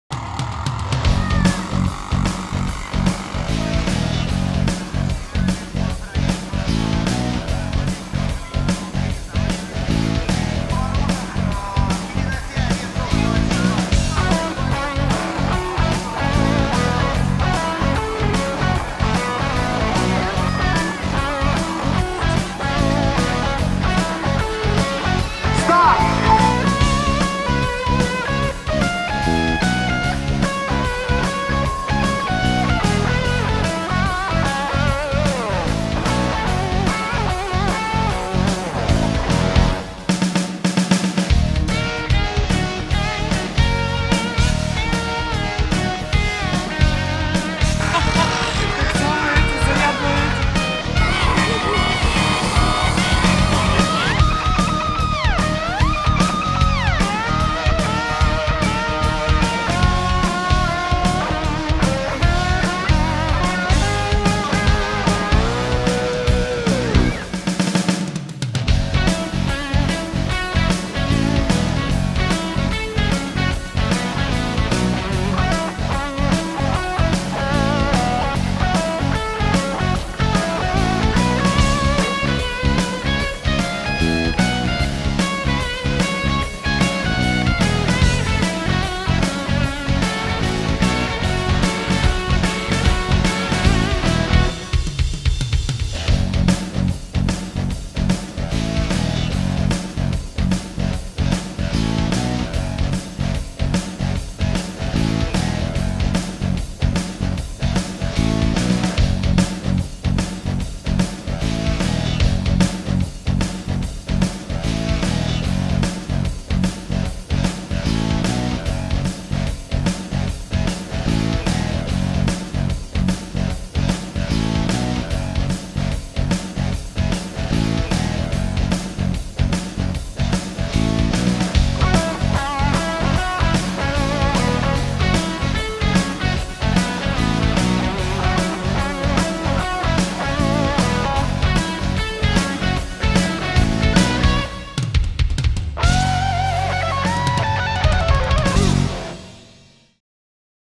Квадро фестиваль 2013
Моря солнца, отличного настроения и драйва было обеспечено всем участникам и зрителям! Каждый из участников получил массу адреналина.